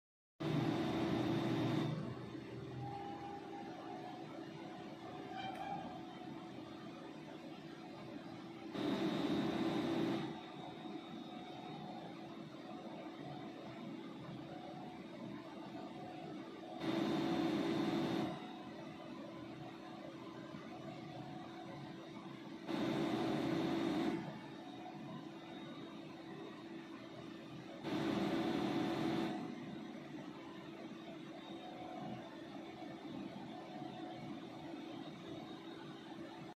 Right Ovarian Mucinous Cystadenoma on Ultrasound | Anday Dani Ki Rasoli | Benign Ovarian Tumor
This female patient presents with pelvic pain history.
On Ultrasound Right adnexal region shows a large cystic mass, multilocular with thin septations; loculations contain low-level internal echoes …. Suggestive of Mucinous Cystadenoma.